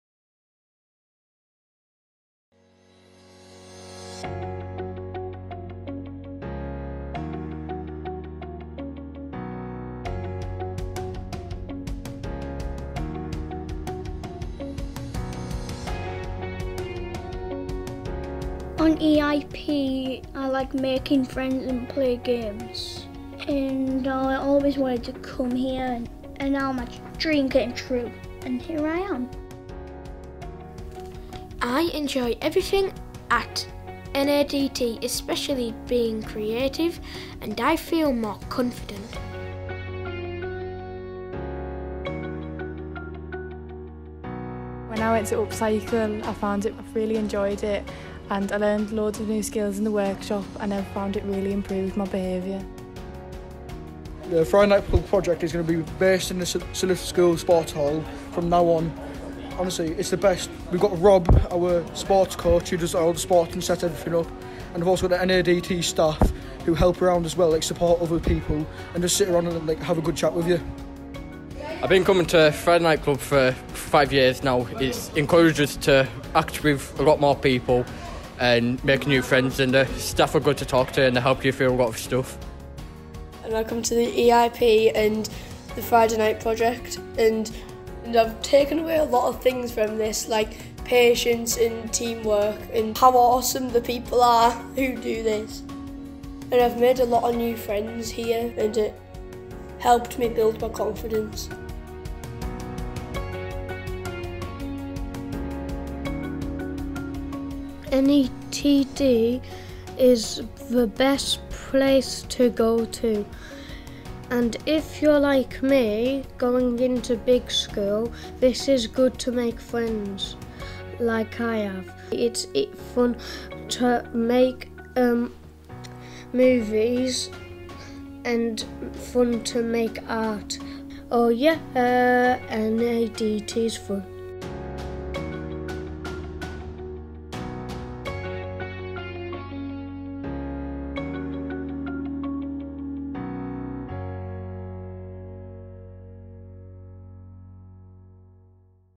NADT - Feedback on our Work
North Allerdale Development Trust (NADT) are a charity based in Wigton, Cumbria.

Here is a short film with feedback from our young people and their schools about the work we do.